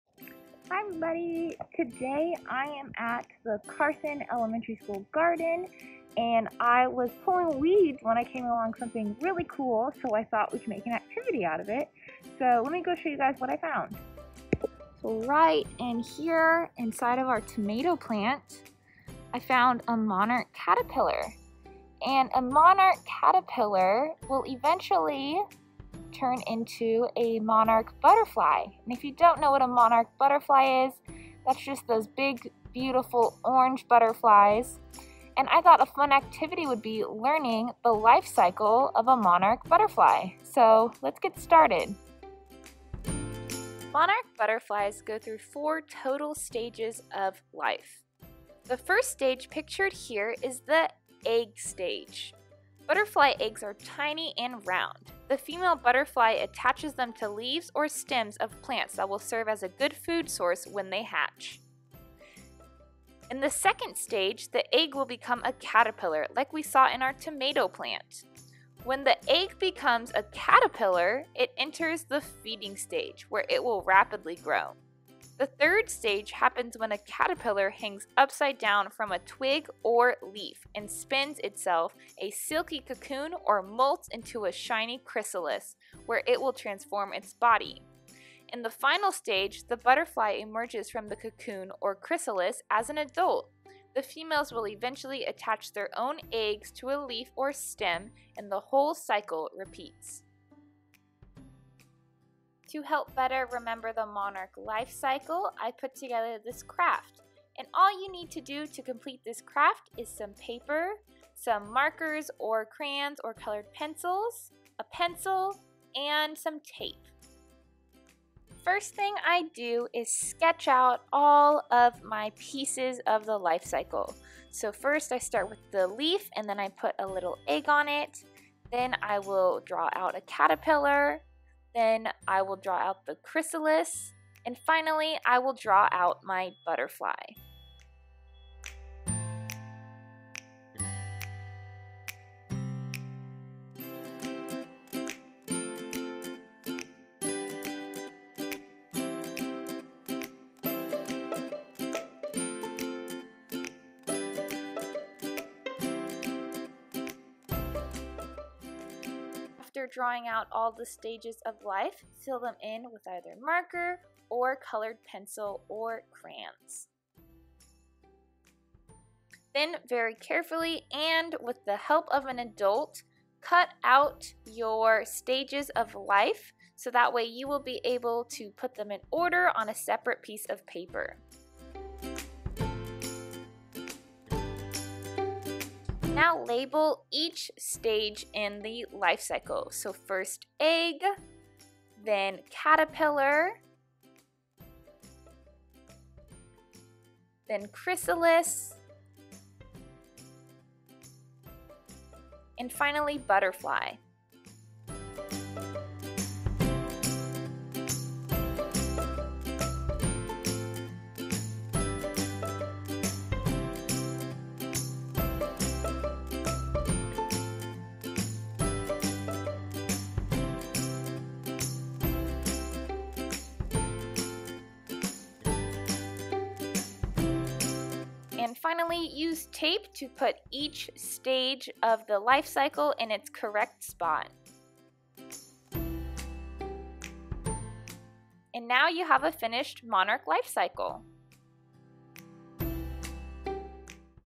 Monarch Butterfly Life Cycle & Craft
Materials needed for the craft are:
1. Paper
2. Markers, colored pencils, or crayons
3. Pencil
4. Tape